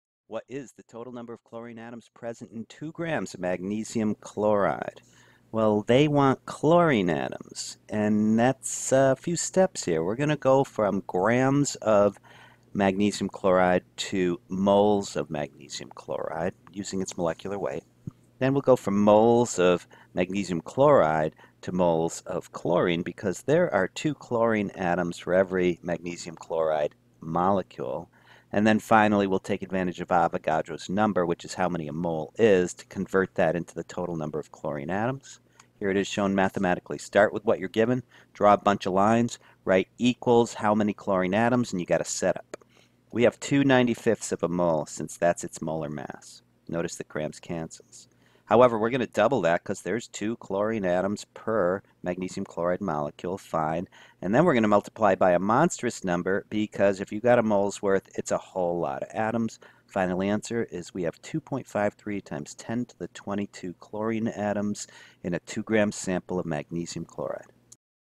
How many Cl atoms are in 2 grams of MgCl2?
59 How many Cl atoms are in 2 grams of MgCl2? Difficulty: Hard